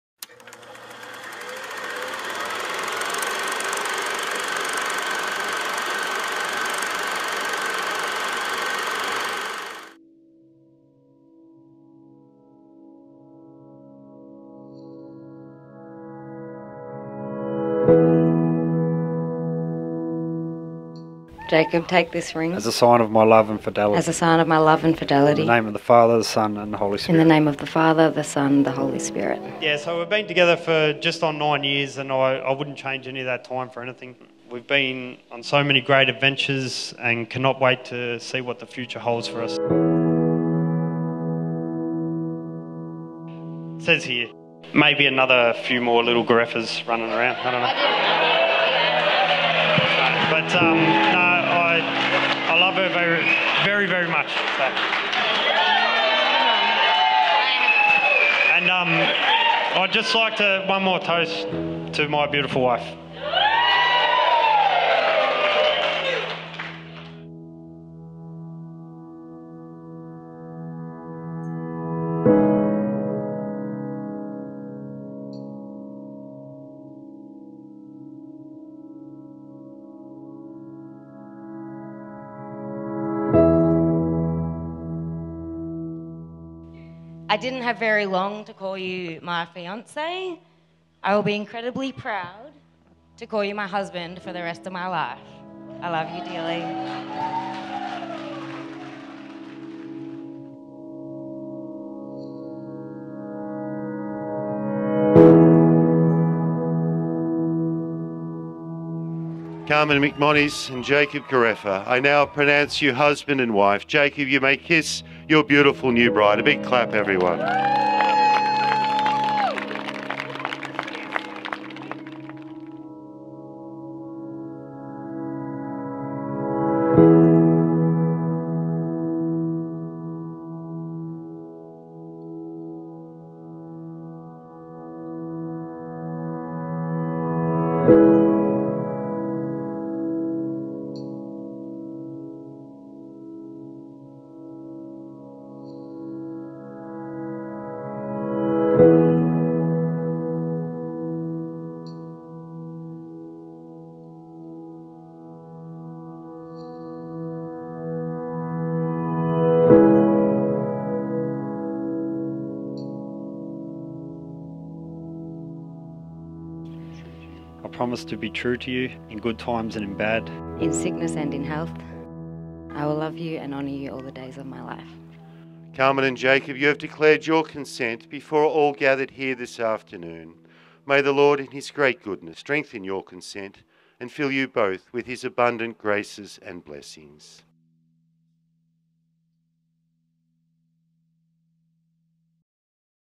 carmen + jakob - one sweet day films - mildura {trailer}
A Friday... in Winter = perfection.  Matching a perfect couple. Loving + kind!!

Wild Awake by Hilary T. Smith

“People are like cities: We all have alleys and gardens and secret rooftops and places where daisies sprout between the sidewalk cracks, but most of the time all we let each other see is is a postcard glimpse of a skyline or a polished square. Love lets you find those hidden places in another person, even the ones they didn't know were there, even the ones they wouldn't have thought to call beautiful themselves.”